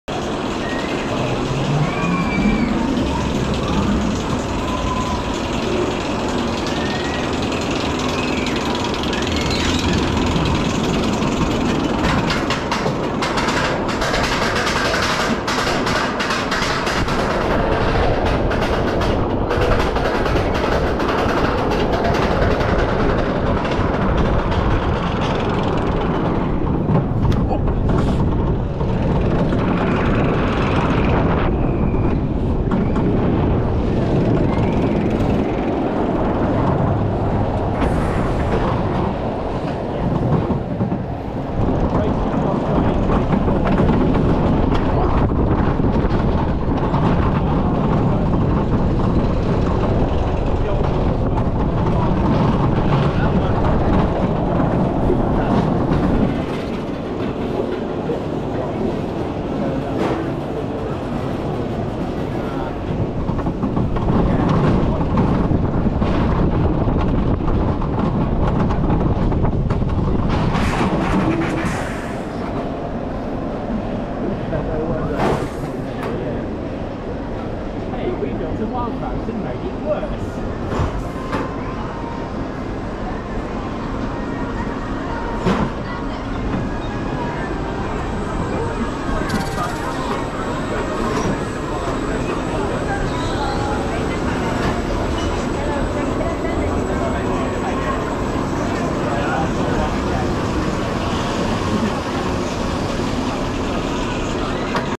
Tulireki, Linnanmaki, Finland 5K60FPS On-ride POV
Take a front row ride on the only Mack E-Motion Coaster in the world, Tulireki at Linnanmaki in Helsinki, Finalnd